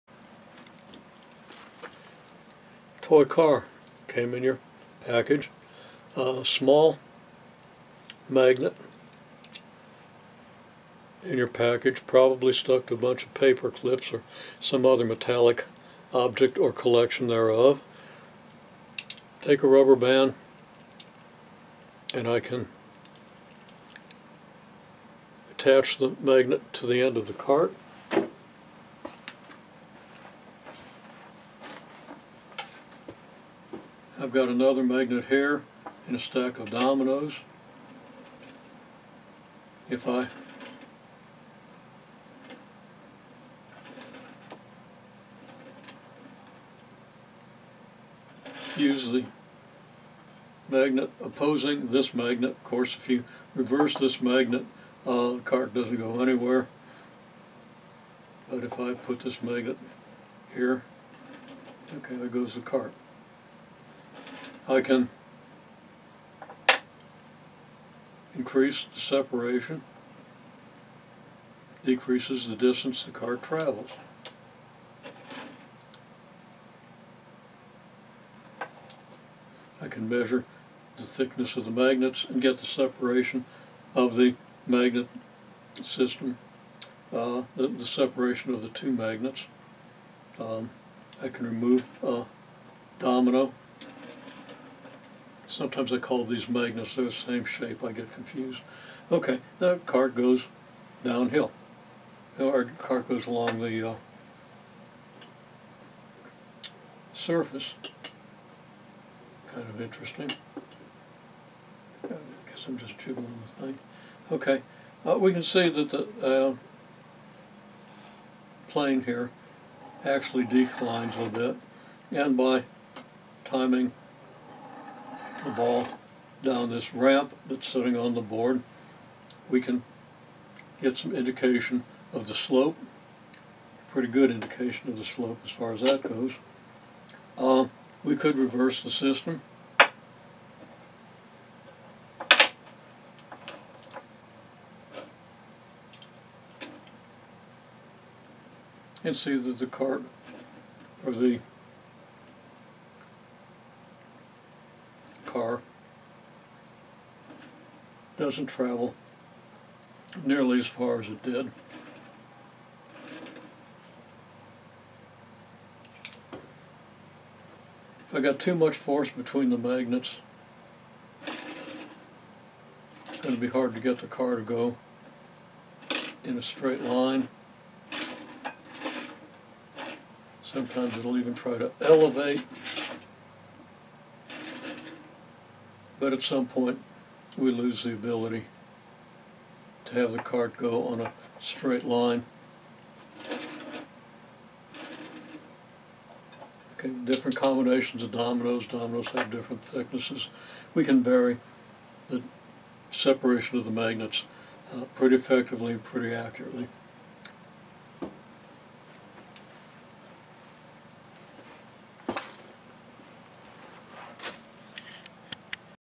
daguniv_2984_demo_hotwheels_car_and_magnets_experiment__4
A demonstration of the apparatus for the hotwheels car and magnets experiment.